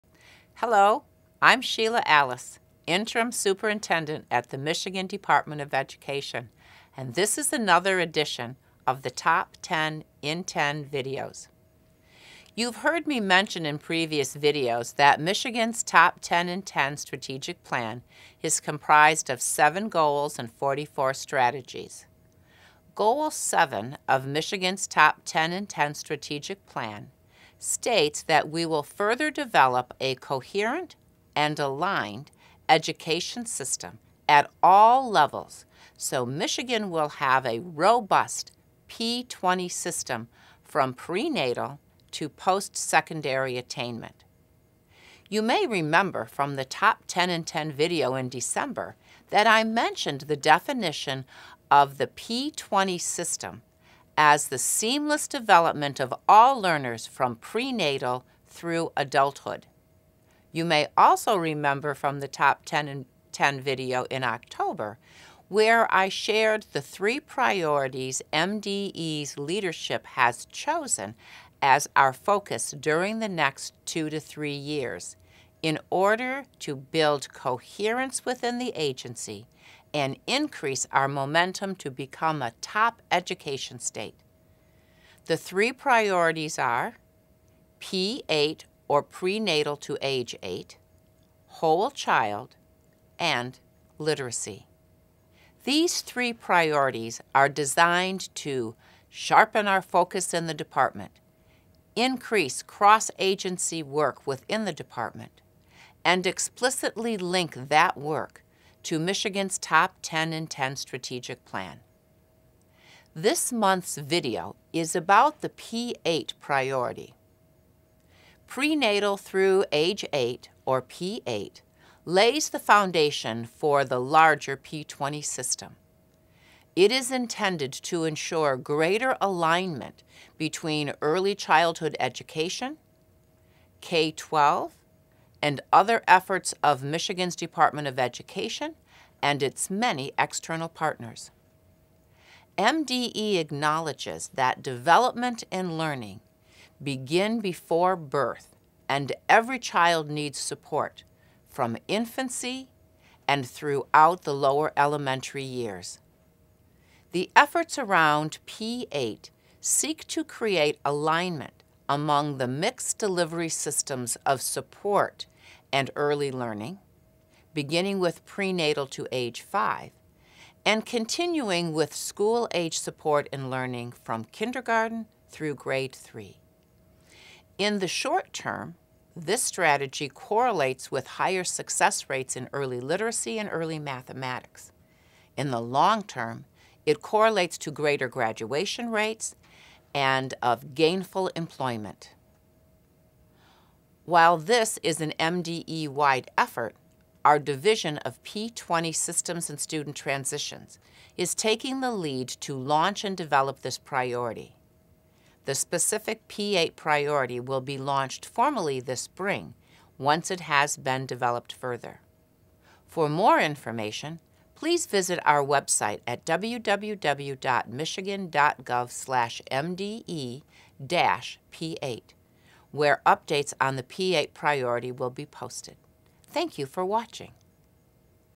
Laying the Foundation For Years of Learning
Development and learning begin before birth and every child needs support. In this month’s Top 10 in 10 video, Interim State Superintendent Sheila Alles shares the importance of the P-8 (Prenatal through age 8) Priority. P-8 lays the foundation for the larger P-20 system. It is intended to ensure greater alignment between early childhood education; K-12; and other efforts of the Michigan Department of Education and its many external partners.

Source: Michigan Department of Education